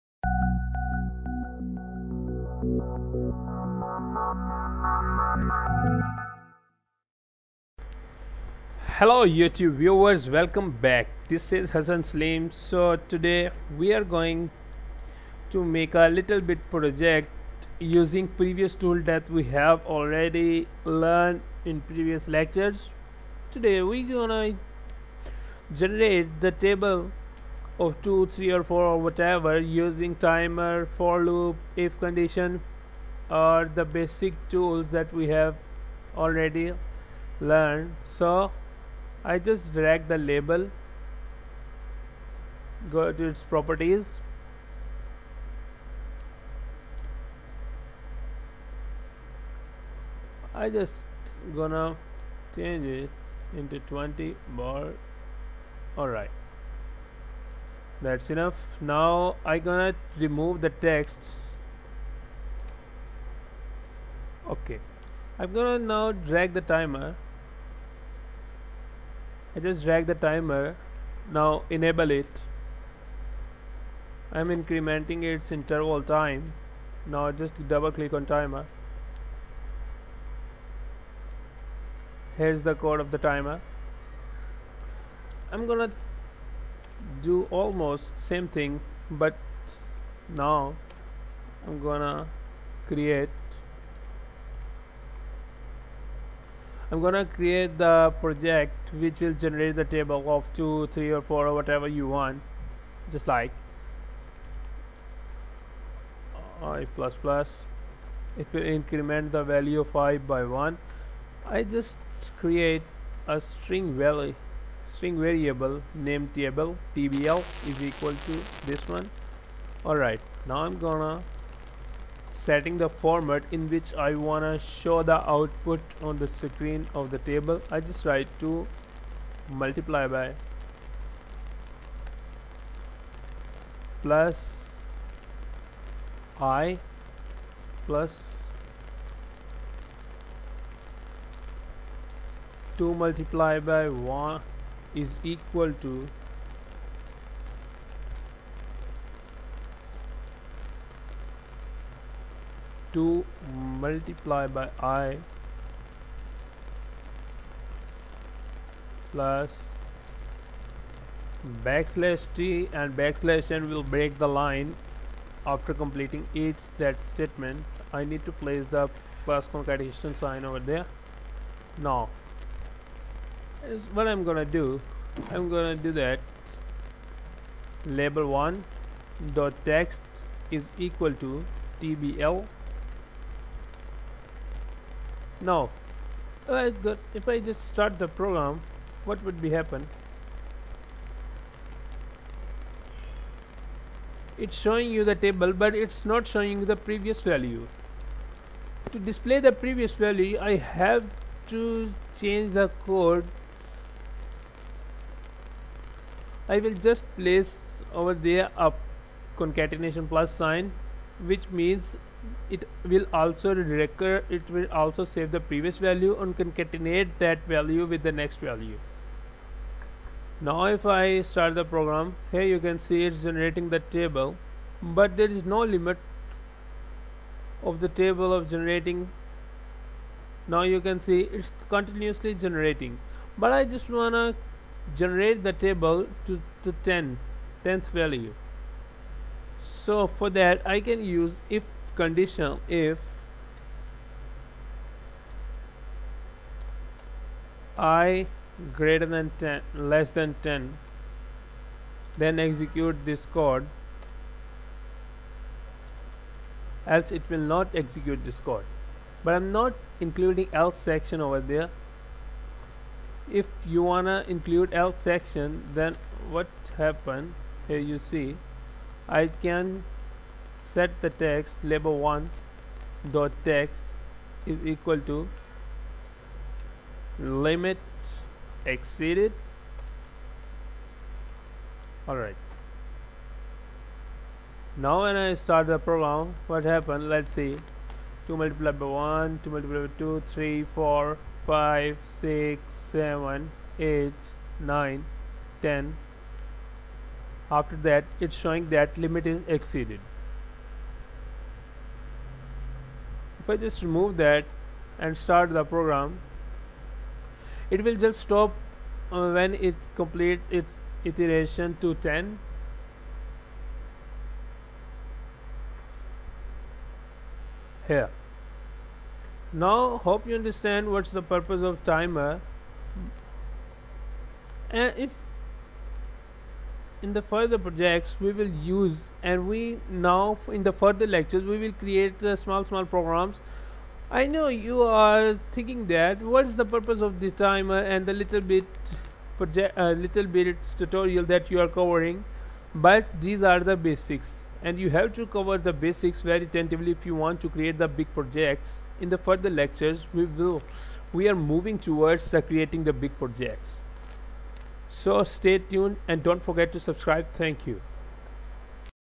C# Windows Form Application Project 01 - 07
In this video you will learn how to make a table generator program using timer in c# visual